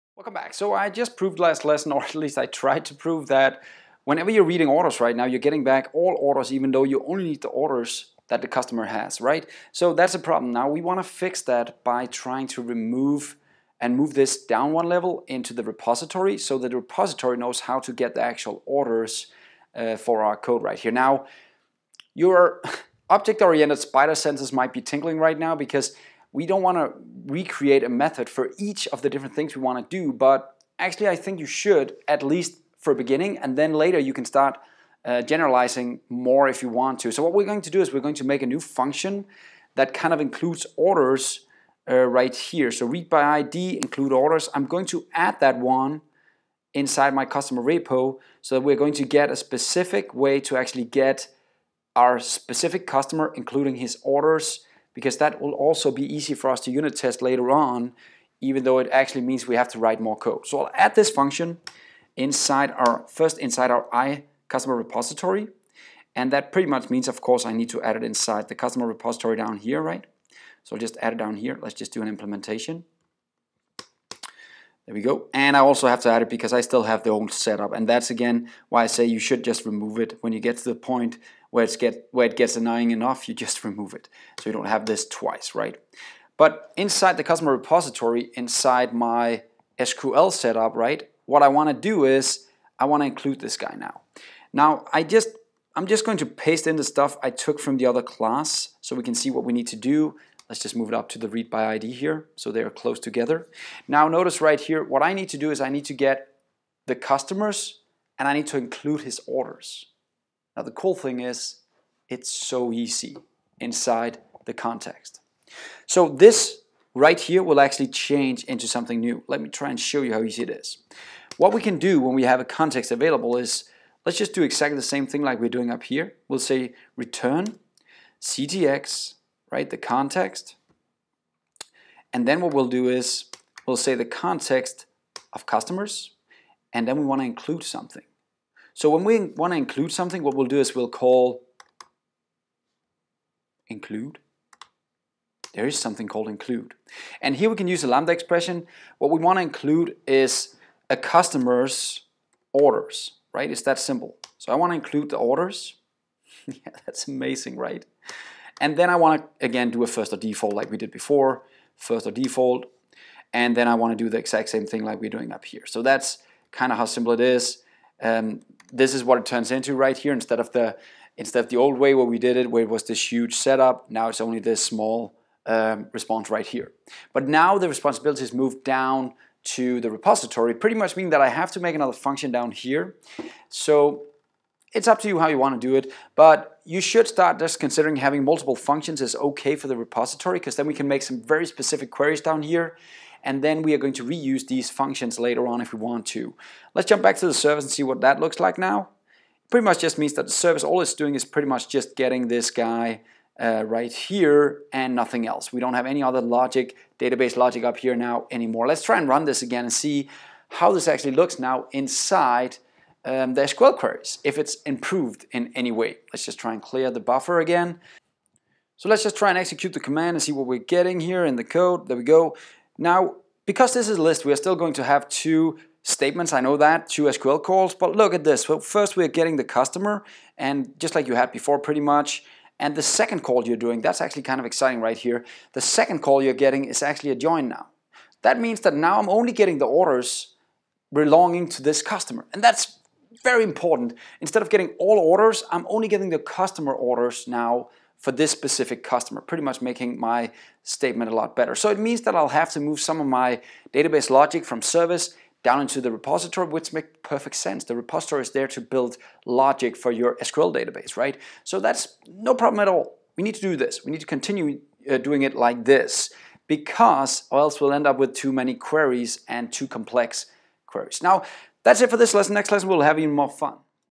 C# Clean Arch Entity Framework Core 2018 | S4P18 | Improving ReadById SQL Queries
The 2018 series 2/5 is a new version of an older series, in the older series from 2017 I use the good old 3 layered Architecture, the 2017 version is available here:

In 3/5 We will be adding a REST API in the .net Core framework and extend a bit on the Clean Architecture: